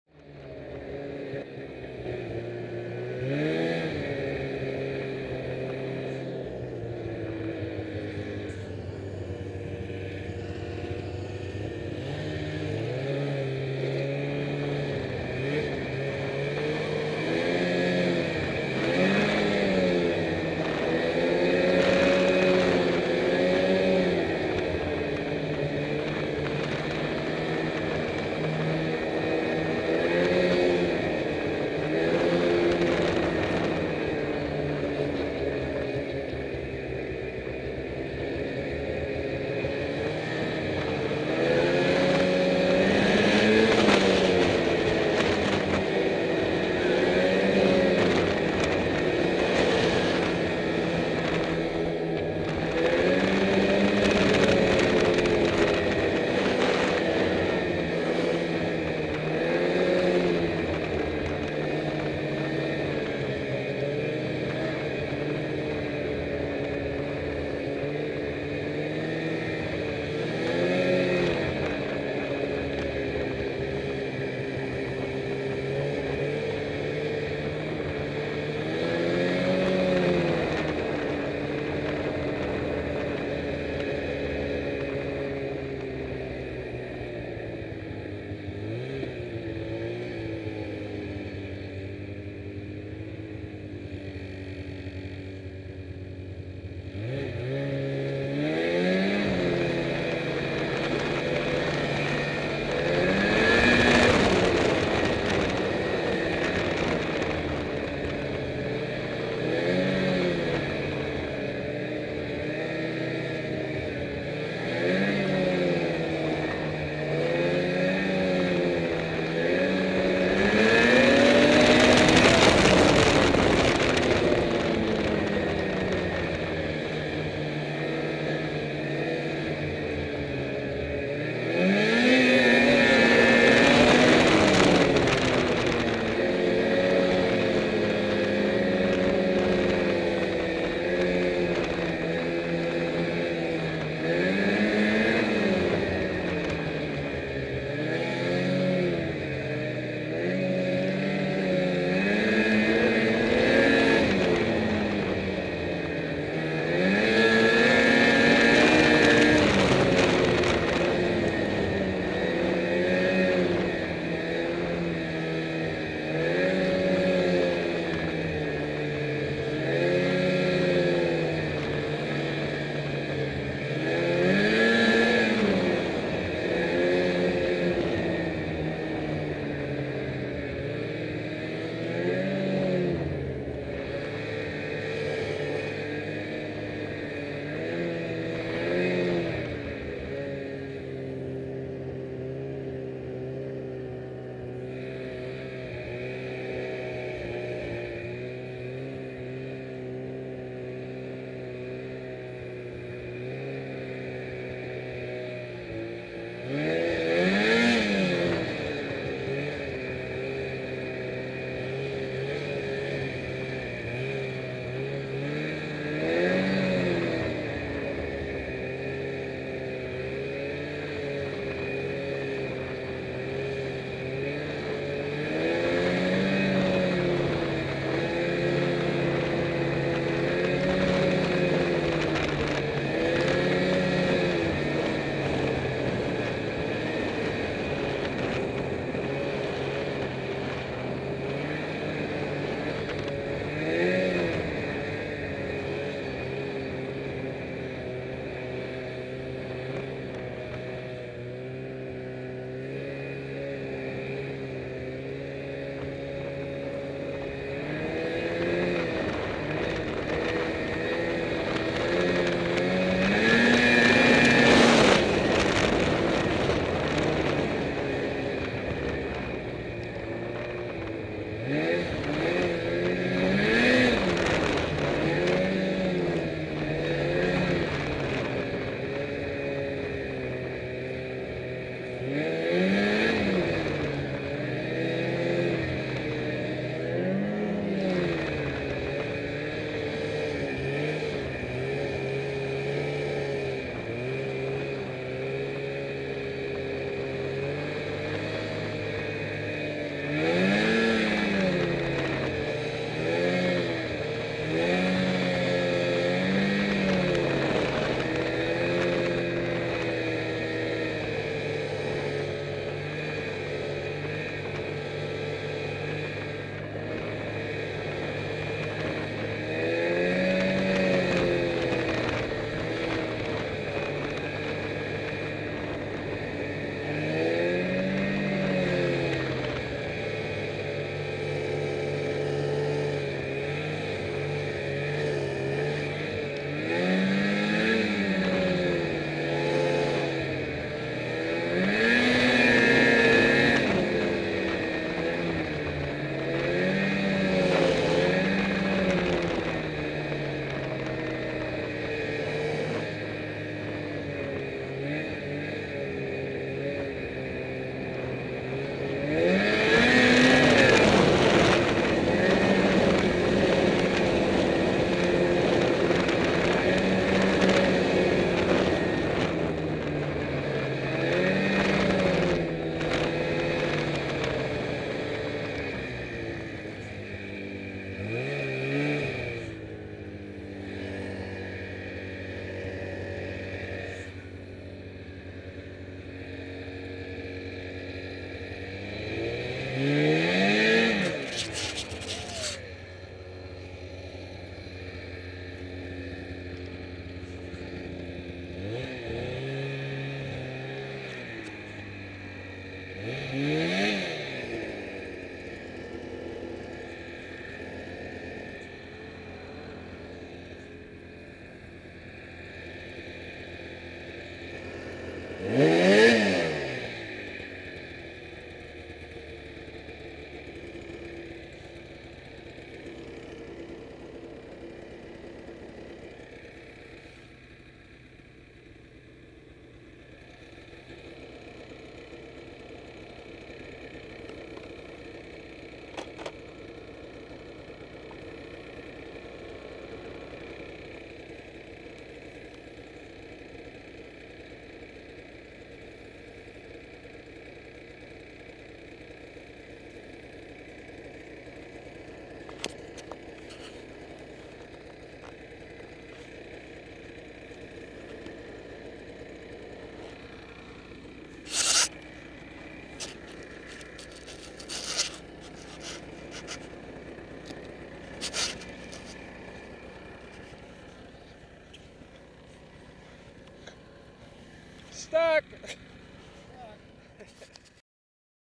Another Test with Contour Camera
Riding around at Stanislaus Meadow near Highway 4, testing a Contour helmet camera.